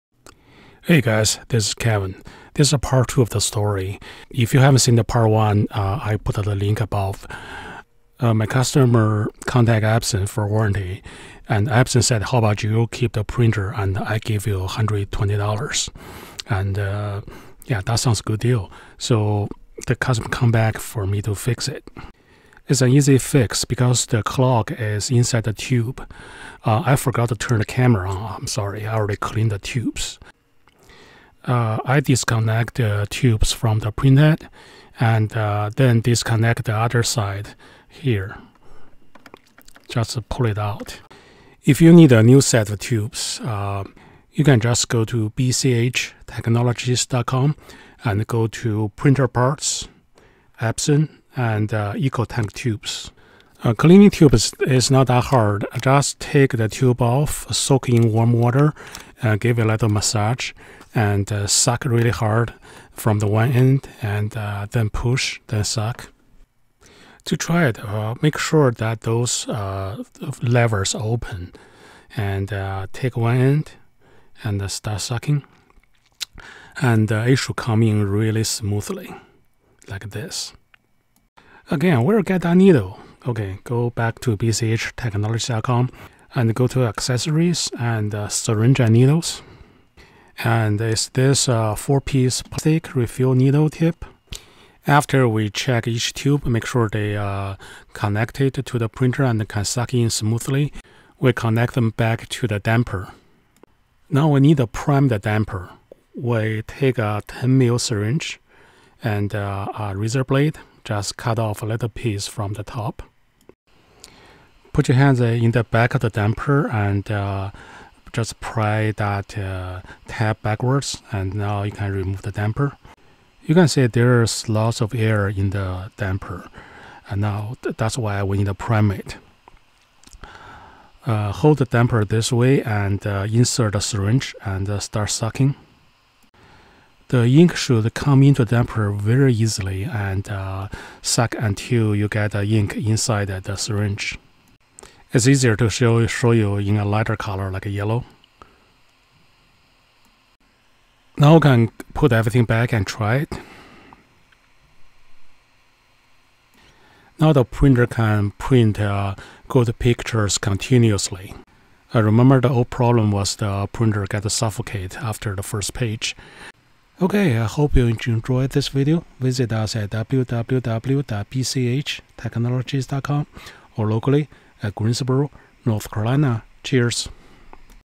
Epson EcoTank Clogging Troubleshoot Part 2 - Unclog Tubes and Prime Dampers
Are you tired of constantly dealing with clogged tubes in your Epson printer? Don't throw it away just yet! This video shows you how to easily fix clogging issues and get your printer back to its top performance. Join us as we take you through the step-by-step process of cleaning the tubes, priming the damper, and ensuring the proper connection of the lines. Don't miss out on this opportunity to save your printer and money. Watch now to learn the solution to your Epson printer woes! The customer returned the printer, and we cleaned the clogs inside the Epson EcoTank tubes.  It turned out that the printer was a demo printer and had been on the shelf for a long time.  We were able to unclog the tube.  The printer was working fine after we primed the damper.

00:00:00 Kevin introduces himself as the speaker.
00:00:02 Part two of the story is introduced.
00:00:04 The link to part one is provided for those who haven't seen it.
00:00:08 Kevin explains how the customer's Epson printer was fixed by cleaning the clogged tubes and priming the damper.